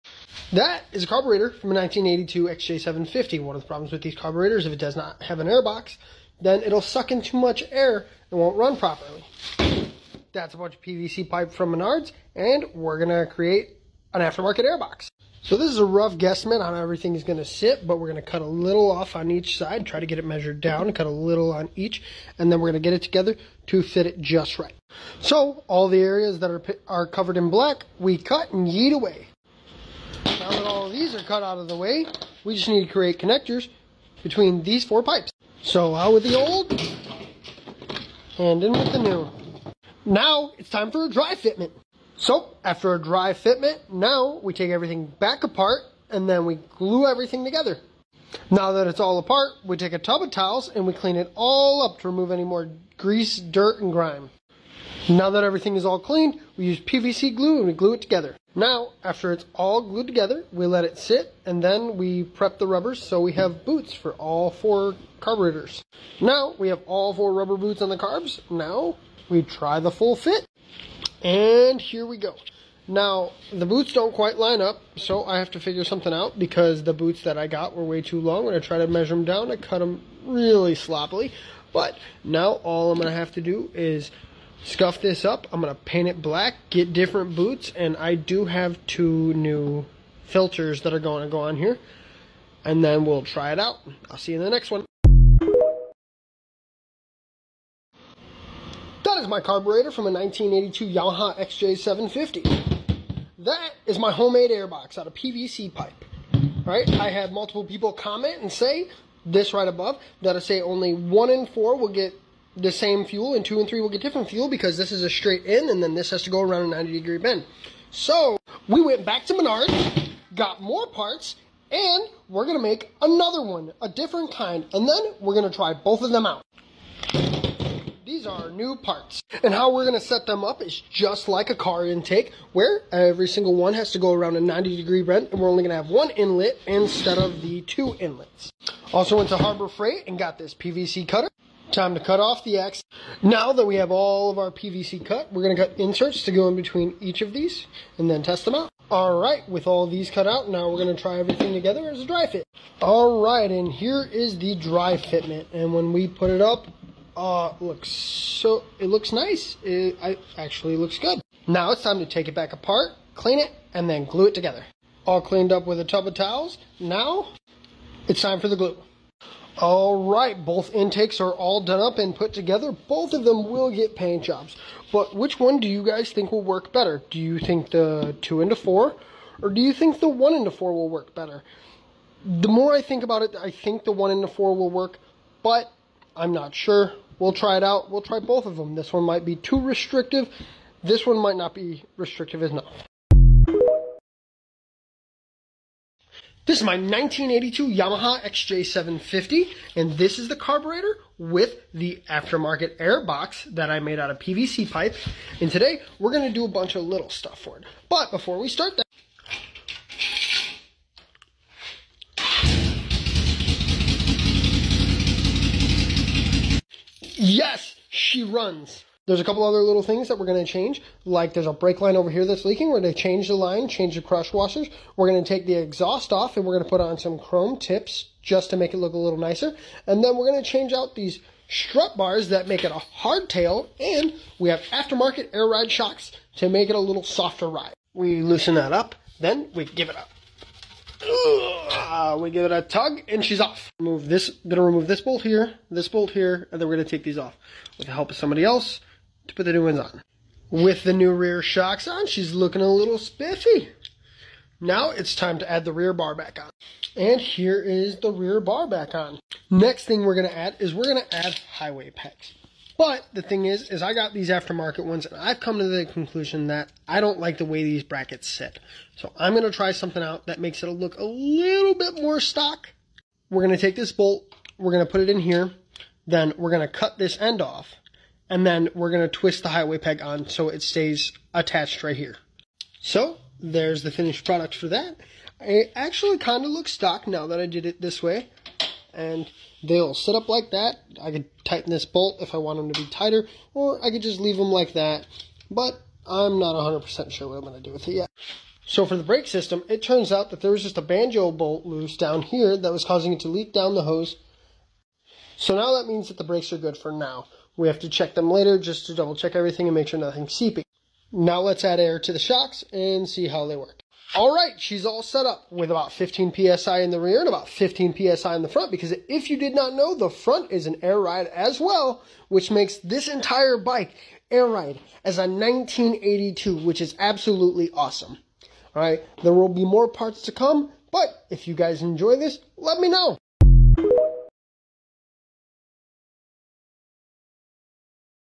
How to make an aftermarket airbox.
this is my 1982 yamamaha xj750 it has no air box and was converted to a hard tail. let's make an air box and convert it back. this works on most 70s 80s and 90s japanese 4 cylender motorcycles.